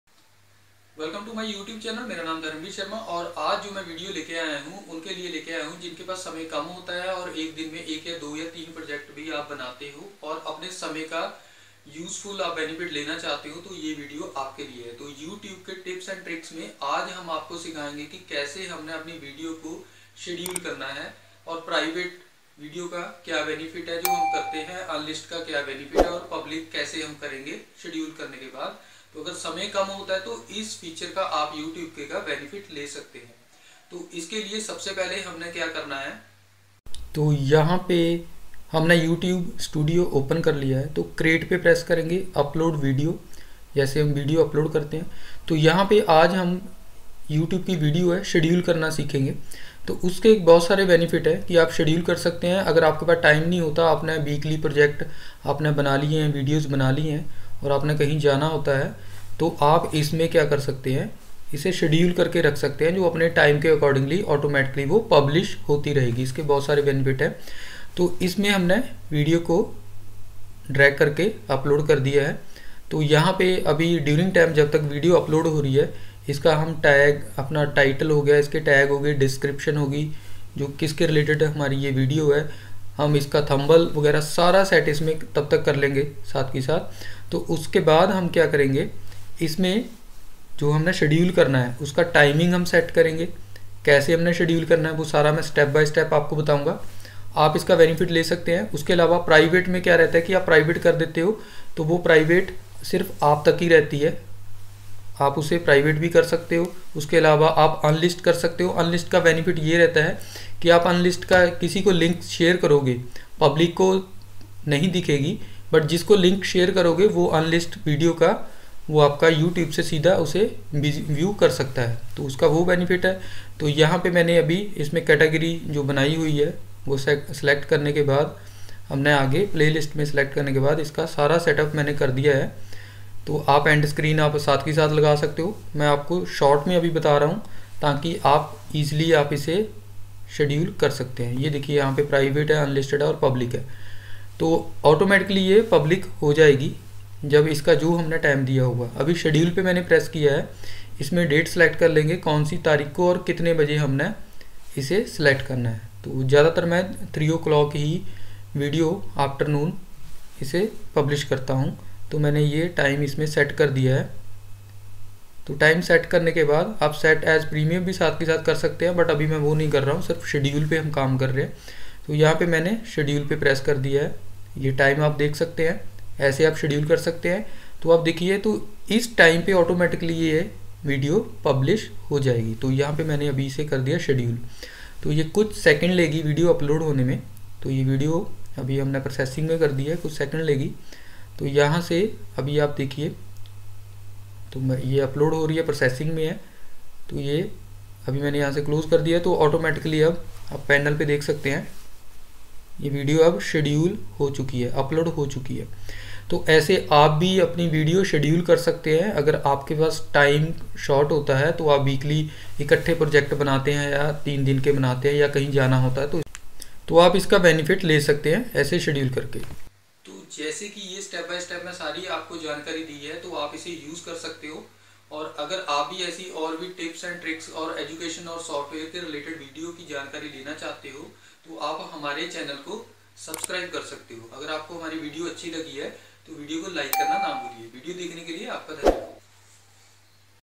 YouTube Schedule Upload Kaise Kare | How to Schedule a YouTube Video | YouTube Updates
How to schedule a youtube Video | Yeh Setting Kro Video Khud Se Upload Ho Jayega

Your Query
1. Does scheduling a YouTube video affect views?
2. When should you schedule a YouTube video?
3. Can you schedule YouTube videos on mobile?